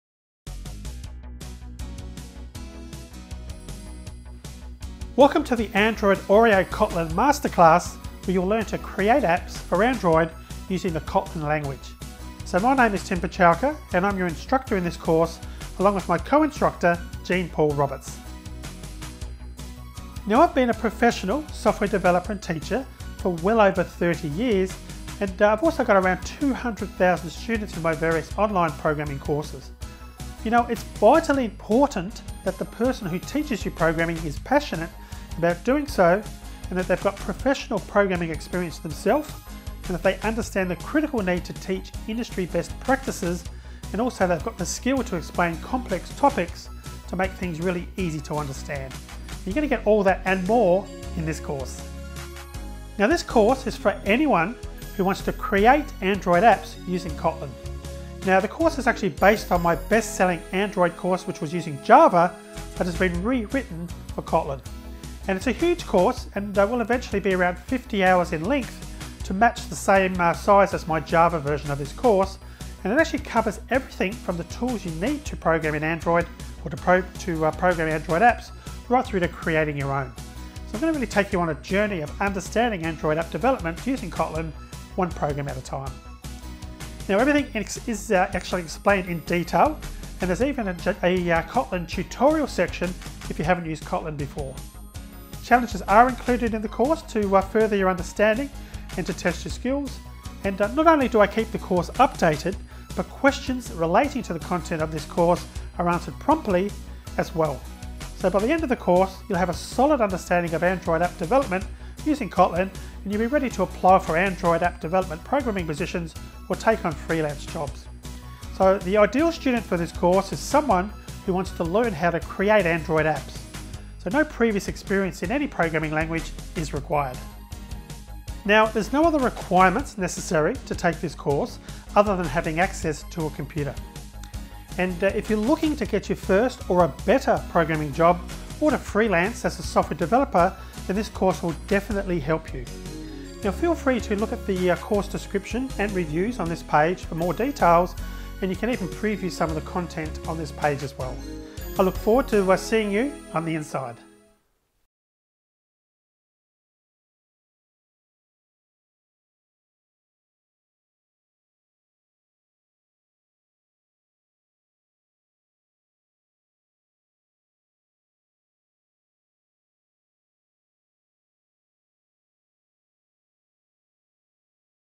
Android App Development Masterclass using Kotlin on Udemy - Official
Welcome to the Android Oreo Kotlin Masterclass! Your instructor for this course is Tim Buchalka along with his co-instructor Jean-Paul Roberts. Tim has well over 30 years of experience in teaching software programming and has over 300,000 students in his various online programming courses. He'll be sharing to you his knowledge, passion, experience and the industry's best practices as he takes you on this journey of understanding Android app development using Kotlin, one program at a time. This course is based on Tim's best selling course which was using Java and has been re-written for Kotlin. This is perfect for anyone who's eager to create Android apps using Kotlin. The course is explained in detail, challenges are included to deepen understanding and test your skills, it's constantly updated and students' questions relating to the course content are answered promptly. Covering from the tools you need to program right through to creating your own, you'll come away with a concrete understanding of Android app development using Kotlin and be ready for more career opportunities relating to Android app development programming. No need to worry if you don't have experience in any programming language. All you need is an access to a computer, an open mind and willingness to learn. Feel free to look through the course description and reviews on this page for more details and we look forward to seeing you on the inside.